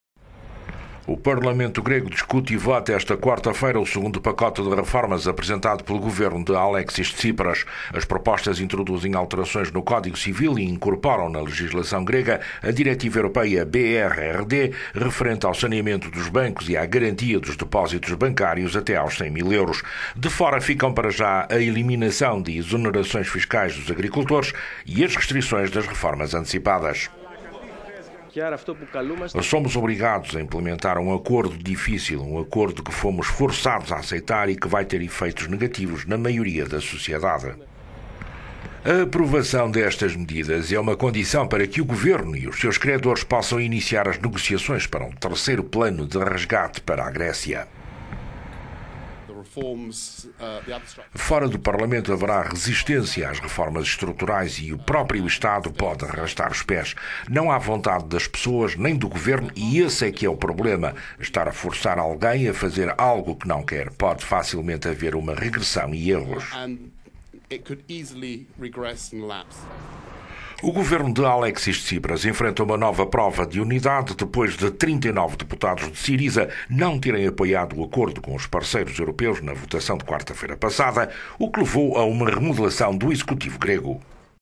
Grécia: Parlamento vota segundo pacote de reformas
O Parlamento grego discute e vota esta quarta-feira o segundo pacote de reformas apresentado pelo governo de Alexis Tsipras.

As propostas introduzem alterações no código civil e incorporam na legislação grega a diretiva europeia BRRD, que referente ao saneamento dos bancos e à garantia dos depósitos bancários até aos 100 mil euros.

De fora ficam, para já, a eliminação de exonerações fiscais dos agricultores e as restrições das reformas antecipadas.